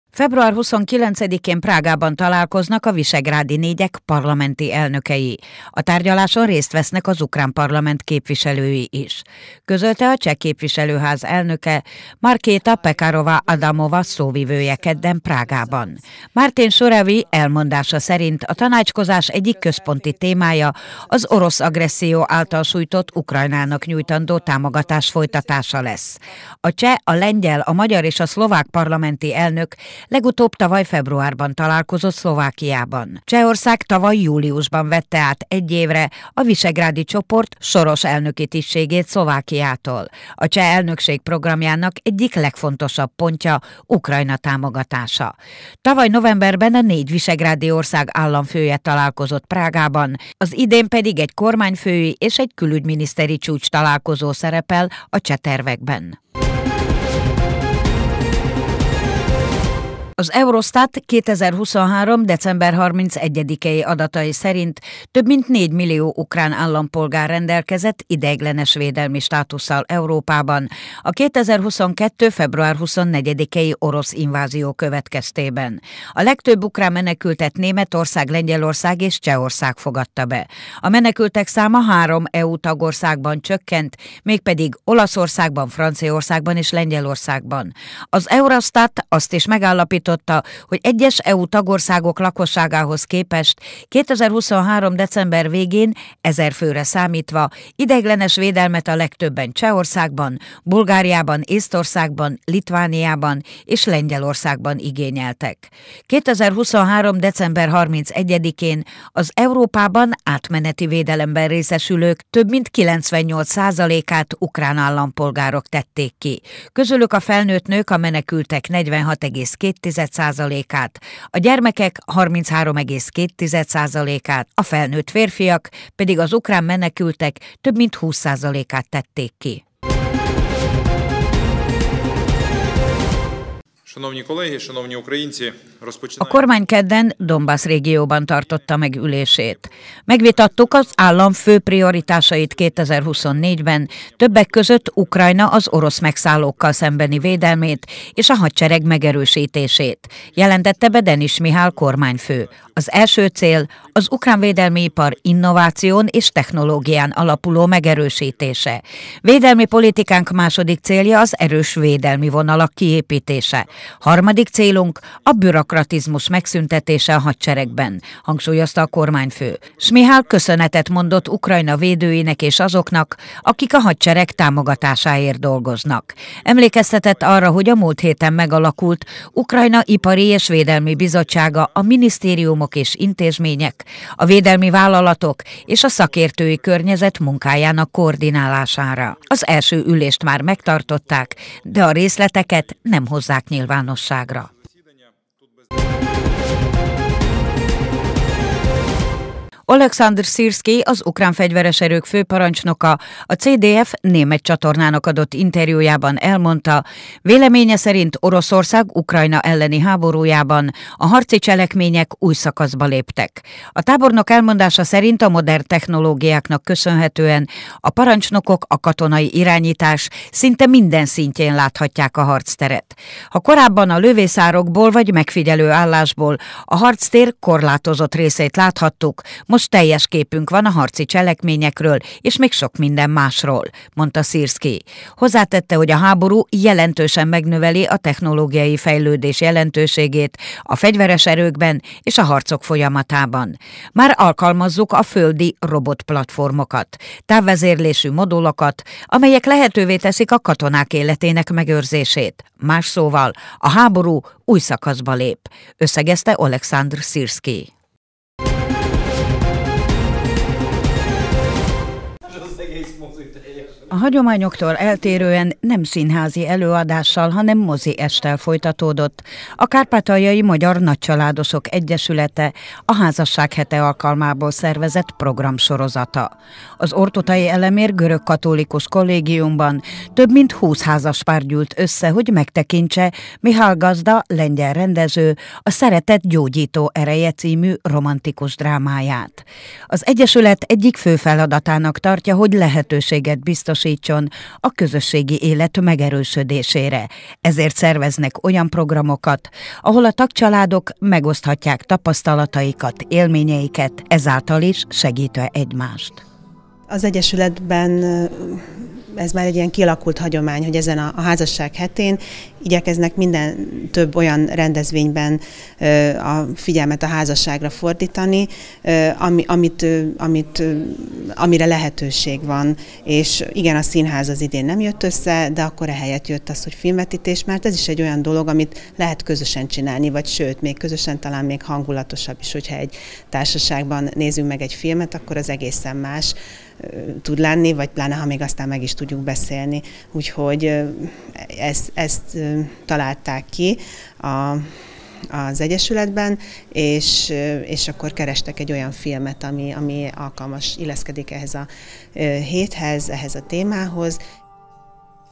Híradó (2024.02.14) Délutáni hírek
- Február végén Prágában találkoznak a visegrádi négyek parlamenti elnökei
- Eurostat: Európában több mint 4 millió ukrán kapott ideiglenes védelmi státuszt
- Smihal megnevezte az állam fő prioritásait 2024-ben
- Szirszkij: új szakaszába lép a háború
- Házaspárok hete- mozi est a KMNE-ben